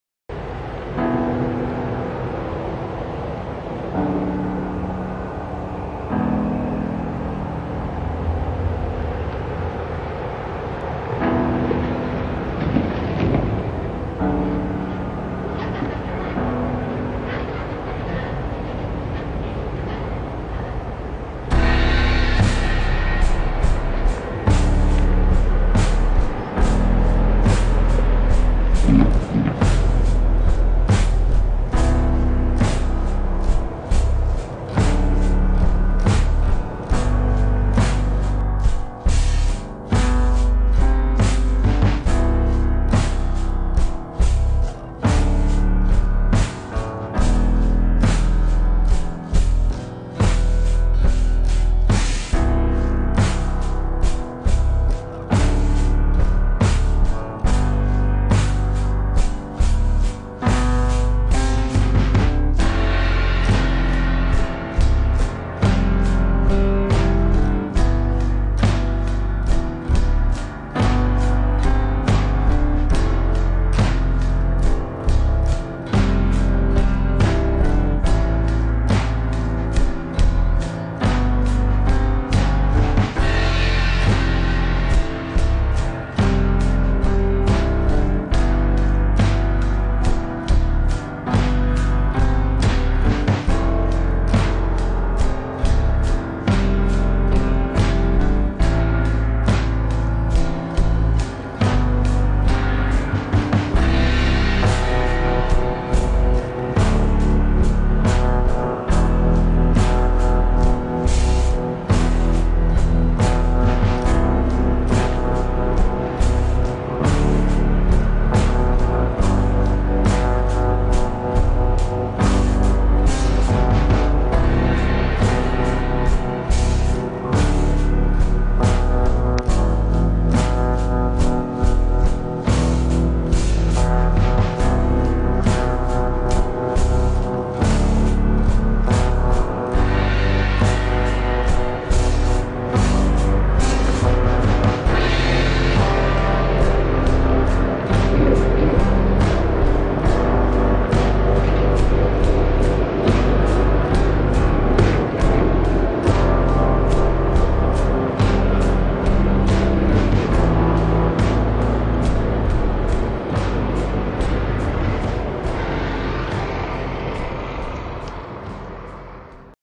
Cheapest Gas Prices location - Honolulu December 2014
$3.21 per gallon. I've been around the Honolulu area and this gas station has the lowest pay by cash gasoline price that I've found. The price will be slightly higher when paying by credit card as shown in this video. The location is the Tesoro gas station on Kapahulu Avenue near Date Street. If you find any gas station that has lower prices for regular unleaded fuel, post in the comment.